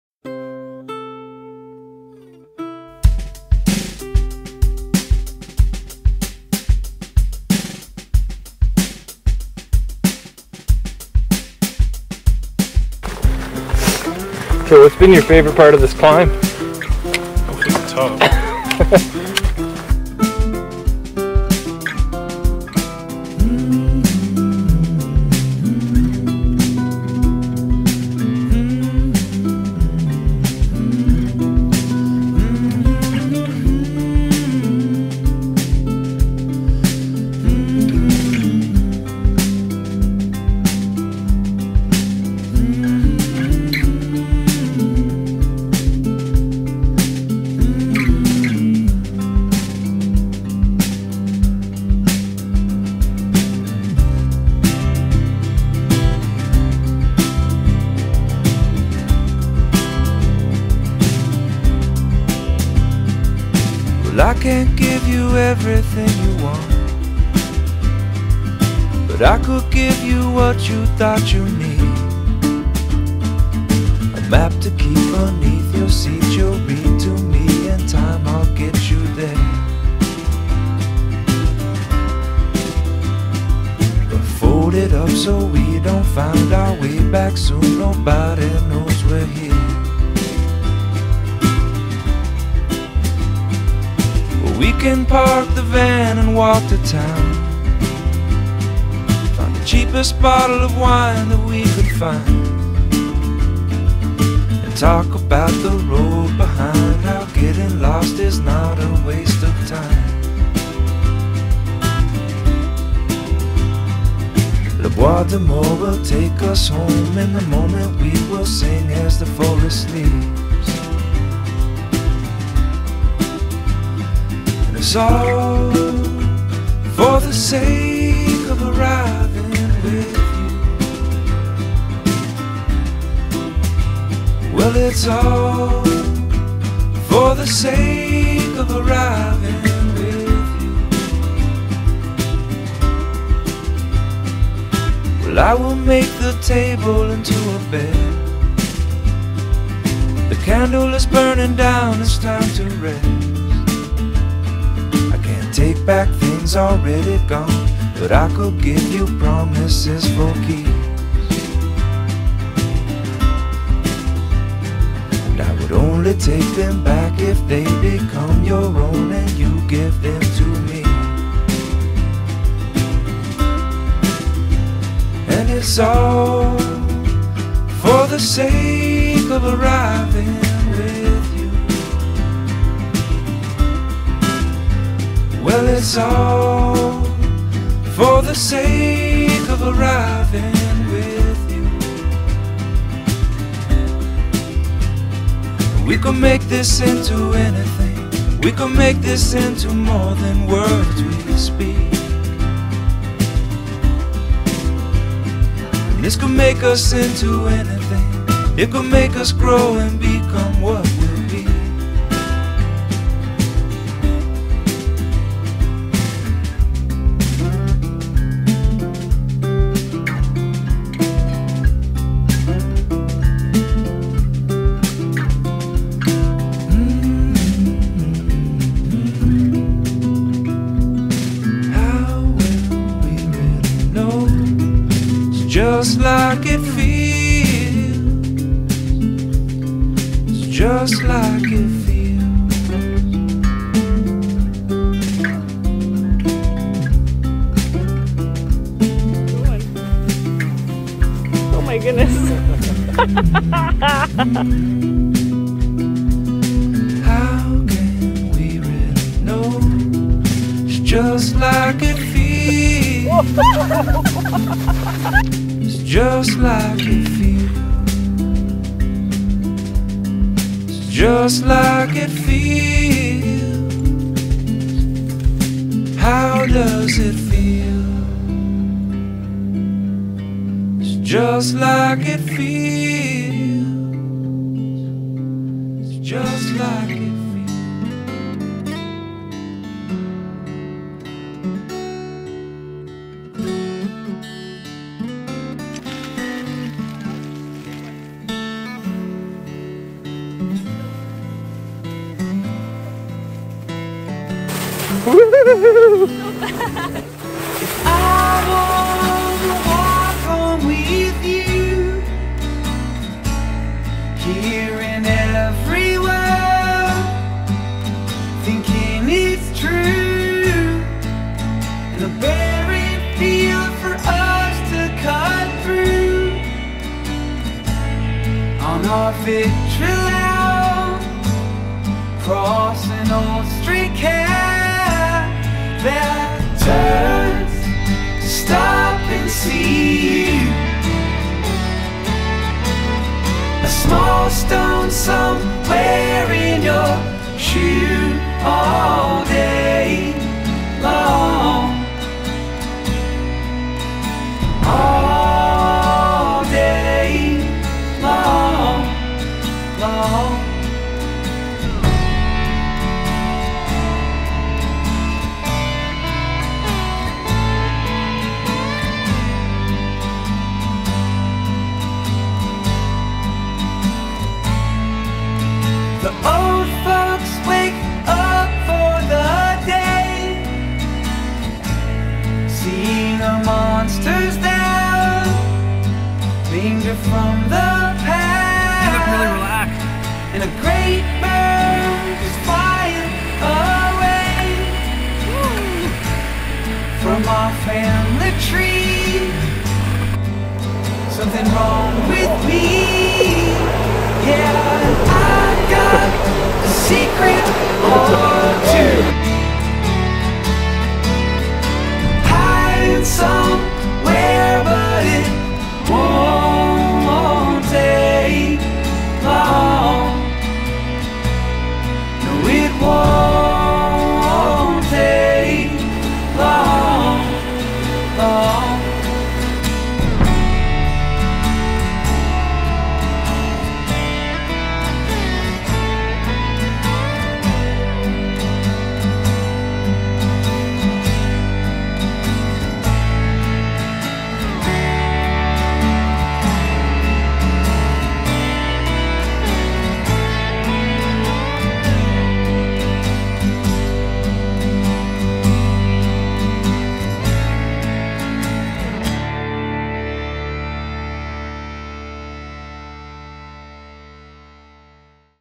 Bob Marshall Packraft 2016: Flathead - Pagoda -Silvertip - NF Sun Loop
8 days of paddling rivers, hiking trails and climbing peaks in the Bob Marshall Wilderness. Highlights include an off-trail ramble over Silvertip (8900'), rafting the South Fork Flathead River and the North Fork Sun River including the lower gorge. 105 miles of adventure.